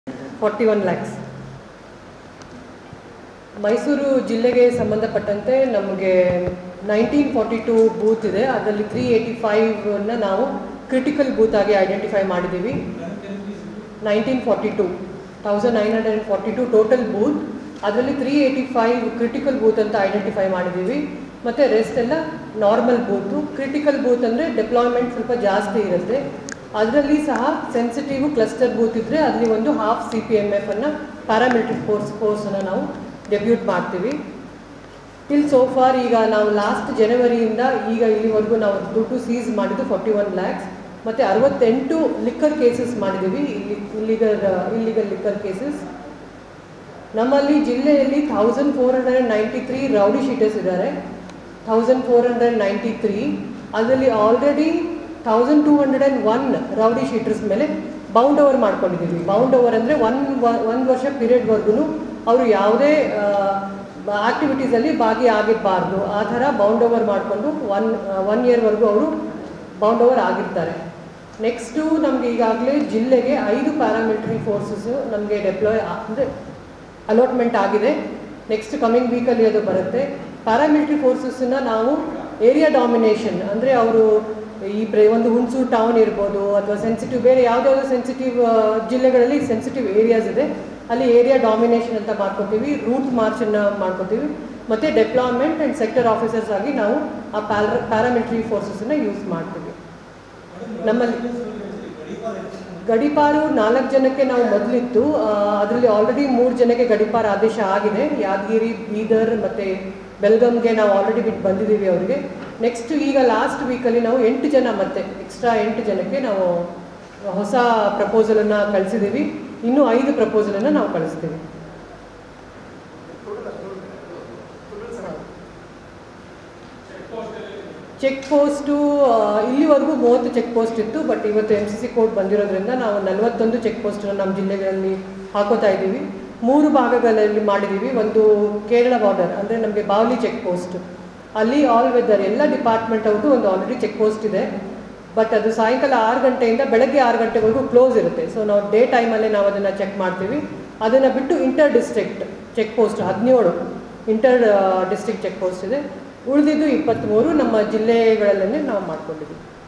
68 ಲಿಕ್ಕರ್ ಕೇಸ್ ದಾಖಲಾಗಿದೆ.. ಮೈಸೂರು ಜಿಲ್ಲಾ ಎಸ್‌ಪಿ ಸೀಮಾ ಲಾಟ್ಕರ್ ಹೇಳಿಕೆ..!
Btv News Kannada : MYSORE : ಮೈಸೂರು ಚೆಕ್ ಪೋಸ್ಟ್​​ನಲ್ಲಿ ಒಟ್ಟು 41 ಲಕ್ಷ ಹಣವನ್ನ ಈಗಾಗಲೇ ಸೀಜ್ ಮಾಡಿದ್ದೇವೆ.. ಮತ್ತು  68 ಲಿಕ್ಕರ್ ಕೇಸ್ ದಾಖಲಾಗಿದೆ.. ಮೈಸೂರು ಜಿಲ್ಲಾ ಎಸ್‌ಪಿ ಸೀಮಾ ಲಾಟ್ಕರ್ ಹೇಳಿಕೆ..!